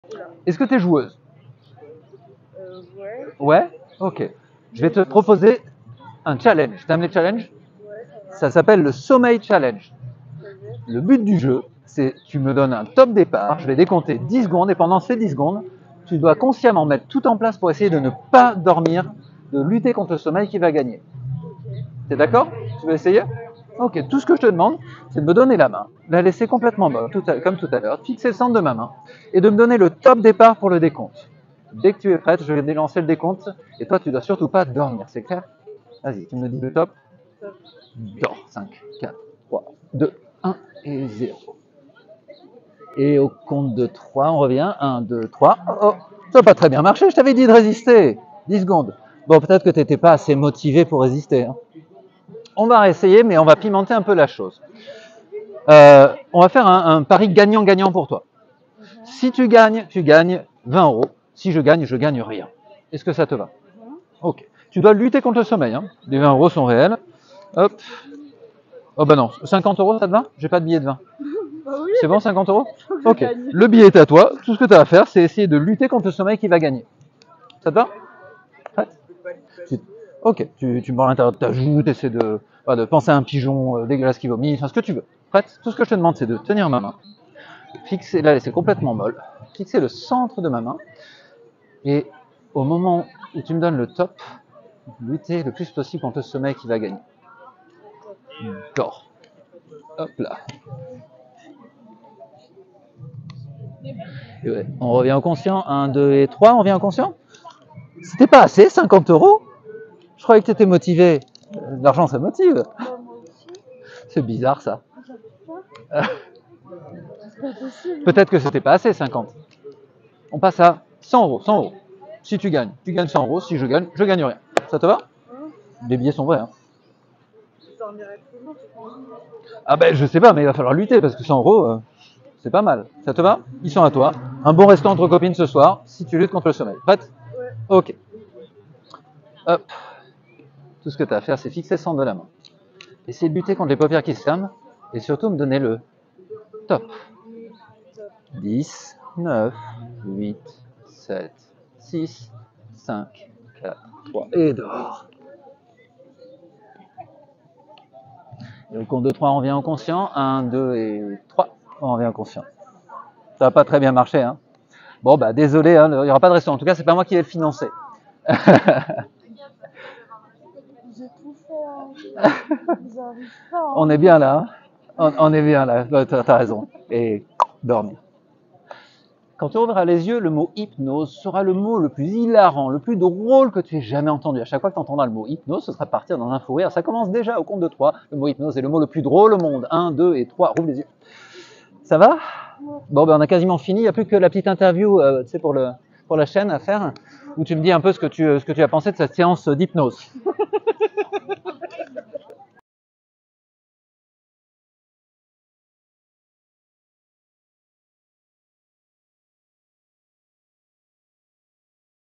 Sleep challenge avec Agathe : try not to sleep for money 🤑 / Street hypnosis
inductions et fractionnement